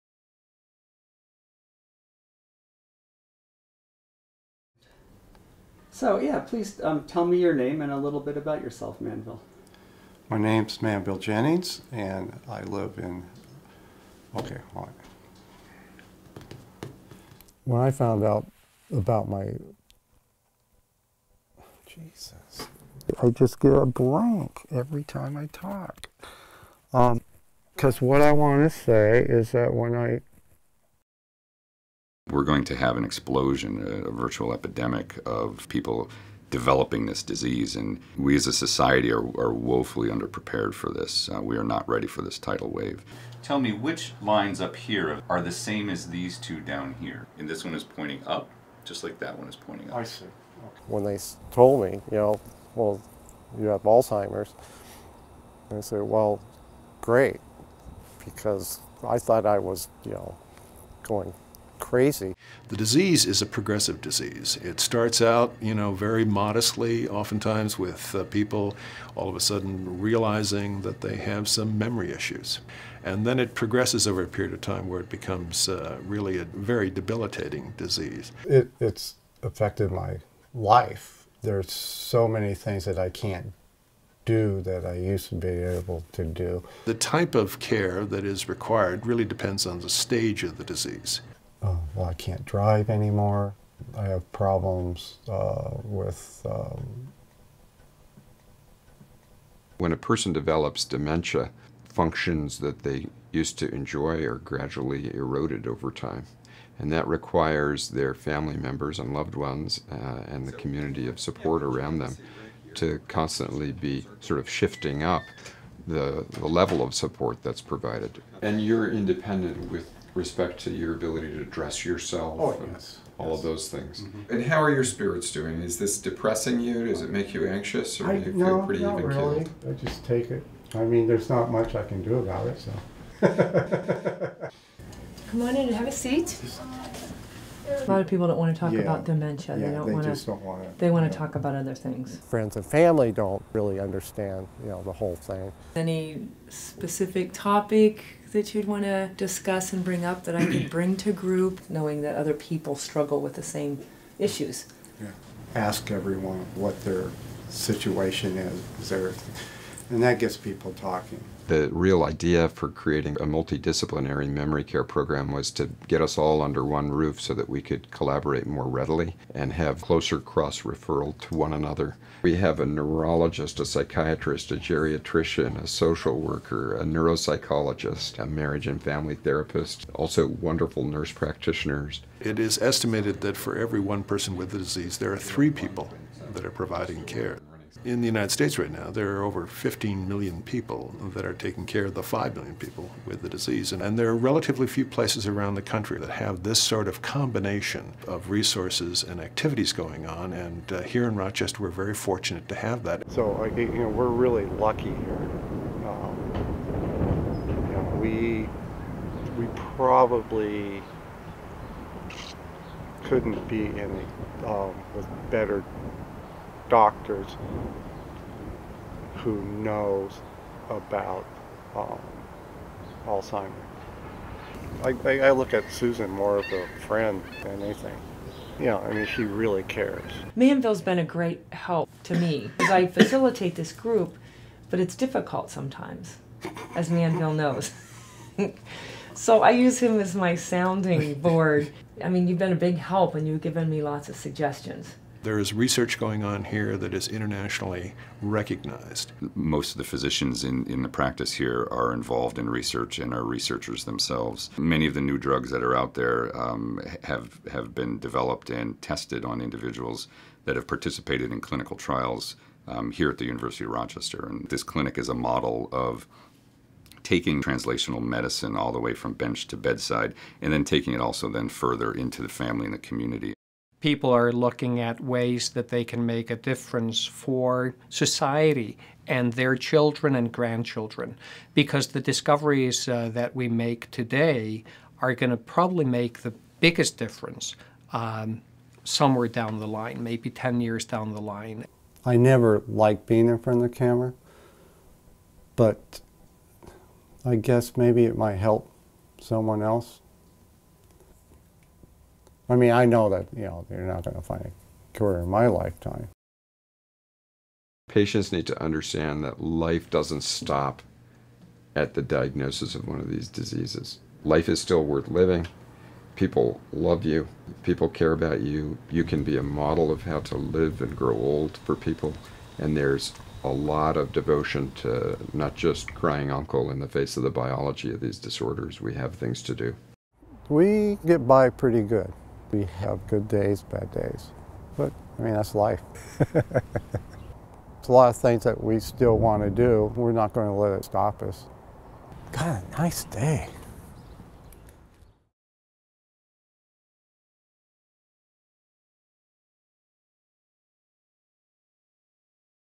Memory Care Program
The University of Rochester Medical Center has opened the doors on a unique, innovative, and multidisciplinary memory care clinic that provides comprehensive evaluation, care, and support for people and families coping with Alzheimer's disease and other causes of progressive memory loss.